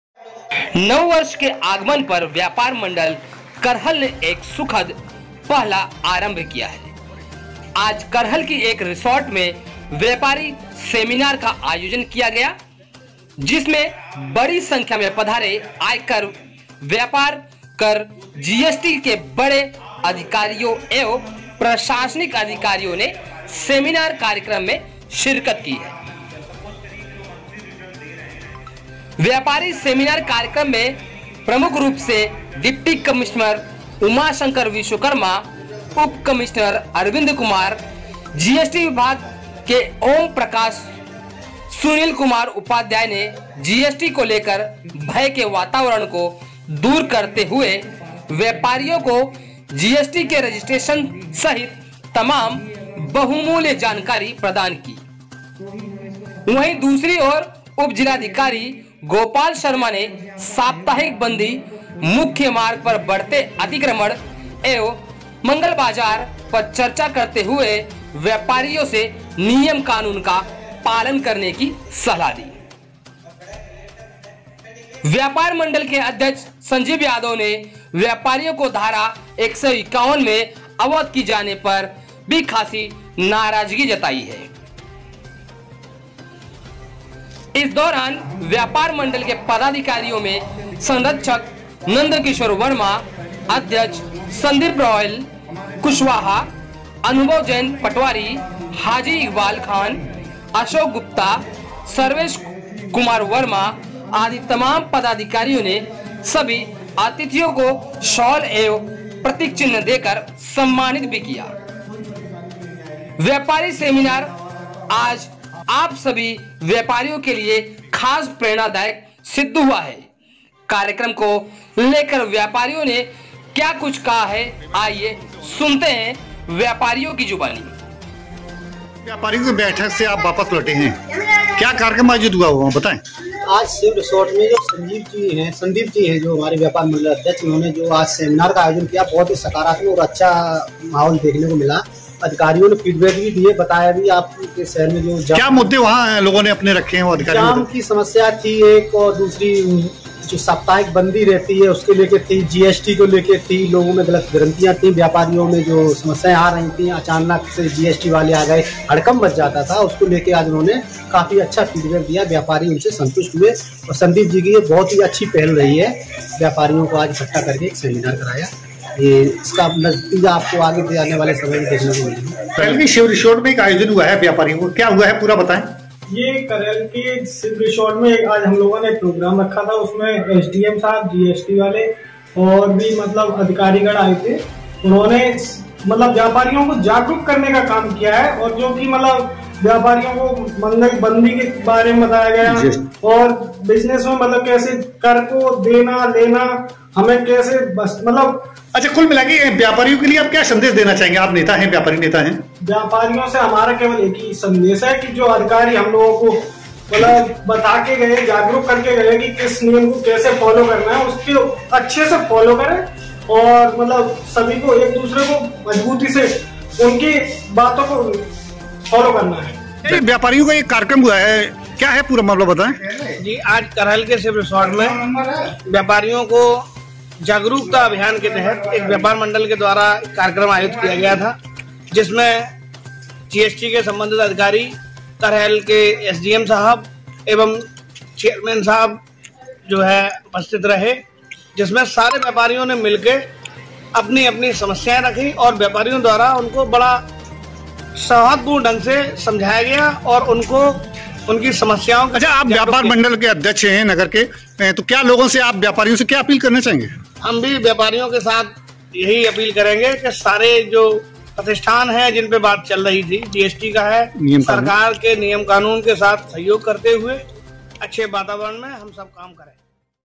करहल में व्यापारी सेमीनार हुआ सम्पन्न
जिला मैनपुरी यूपी
रिपोर्टर रामकिशोर वर्मा

स्लग :--- करहल में व्यापारी सेमीनार हुआ सम्पन्न

अधिकारियों ने व्यापारियों को किया जागरूक

व्यापार मंडल करहल ने अतिथियों को किया सम्मानित

एंकर: नववर्ष के आगमन पर व्यापार मंडल करहल ने एक सुखद पहला आरंभ की है आज करहल की एक रिसोर्ट में व्यापारी सेमिनार का आयोजन किया गया जिसमें बड़ी संख्या में पधारे आयकर व्यापार कर जीएसटी के बड़े अधिकारियों एवं प्रशासनिक अधिकारियों ने सेमिनार कार्यक्रम में शिरकत की है

व्यापारी सेमिनार कार्यक्रम में प्रमुख रूप से डिप्टी कमिश्नर उमाशंकर विश्वकर्मा उप कमिश्नर अरविंद कुमार जीएसटी विभाग के ओम प्रकाश सुनील कुमार उपाध्याय ने जीएसटी को लेकर भय के वातावरण को दूर करते हुए व्यापारियों को जीएसटी के रजिस्ट्रेशन सहित तमाम बहुमूल्य जानकारी प्रदान की वहीं दूसरी ओर  उप जिलाधिकारी गोपाल शर्मा ने साप्ताहिक बंदी  मुख्य मार्ग पर बढ़ते अतिक्रमण एवं मगल बाजार पर चर्चा करते हुए व्यापारियों से नियम कानून का पालन करने की सलाह दी
 व्यापार मंडल के जिलाध्यक्ष संजीव यादव ने व्यापारियों को धारा 151 में अवध की जाने पर भी खासी नाराजगी जताई है

इस दौरान व्यापार मंडल के पदाधिकारियों में संरक्षक नंदकिशोर वर्मा अध्यक्ष संदीप रॉयल कुशवाहा अनुभव जैन पटवारी हाजी इकबाल खान अशोक गुप्ता सर्वेश कुमार वर्मा आदि तमाम पदाधिकारियों ने सभी अतिथियों को शाल एवं प्रतीक चिन्ह देकर सम्मानित किया है व्यापारी सेमिनार आज आप सभी व्यापारियों के लिए खासा प्रेरणादायक सिद्ध हुआ है कार्यक्रम को लेकर व्यापारियों ने क्या कुछ कहा है आइए सुनते हैं उनकी जुबानी

वाइट : संदीप कुशवाहा अध्यक्ष व्यापार मंडल करहल

वाइट :--- अनुभव जैन पटवारी (अध्यक्ष) युवा व्यापार मंडल

बाइट: जितेंद्र कुमार डिंपल (व्यापारी)